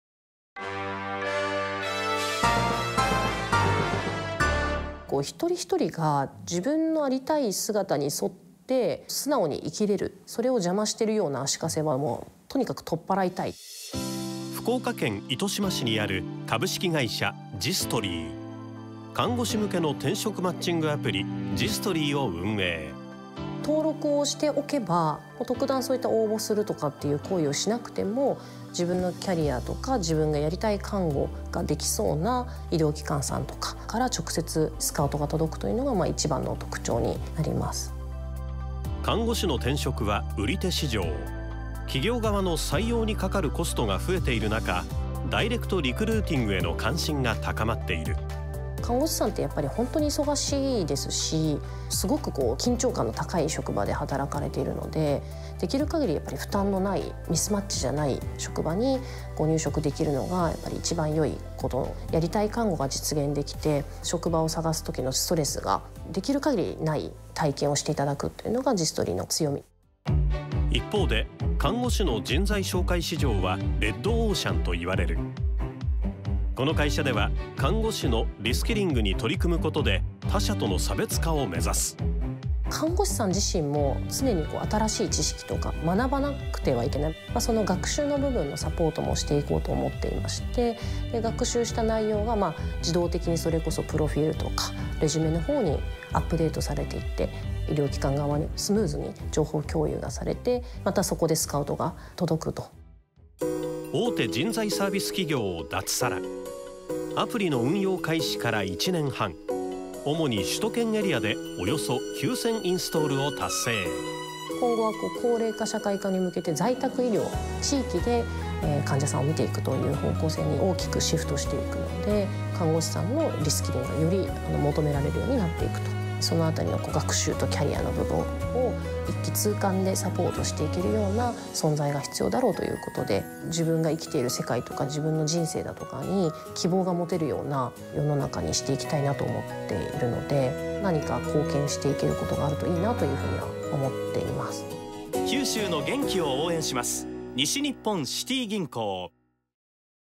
志、情熱企業｜2024年3月30日 株式会社thestory（糸島市二丈町）
【学習支援とスキルの見える化で理想のキャリアが描ける社会へ。看護師向け転職マッチングアプリを運営する企業。】